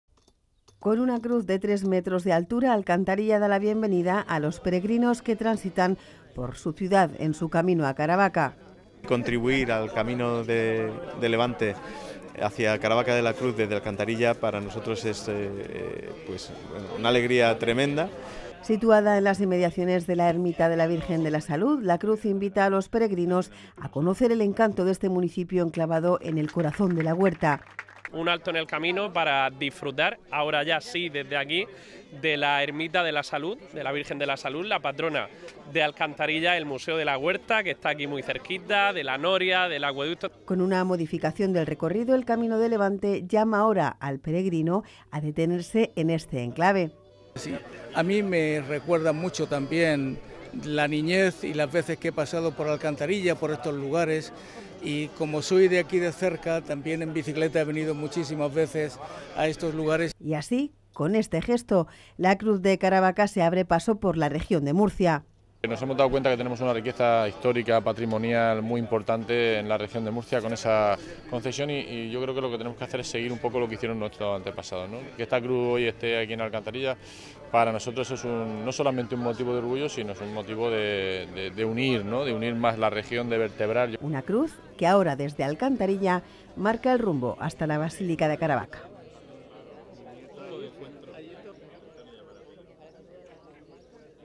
23/07/2024 Una Cruz de Caravaca, de tres metros de altura, marca el punto que pasa por Alcantarilla.
Desde este martes una gran Cruz de Caravaca, de tres metros de altura, marca el punto del Camino de la Vera Cruz que pasa por Alcantarilla.  La ha bendecido el Obispo de la Diócesis de Cartagena, Monseñor José Manuel Lorca Planes. Una cruz que invita a hacer un alto en el camino para conocer también el patrimonio histórico y cultural del este municipio.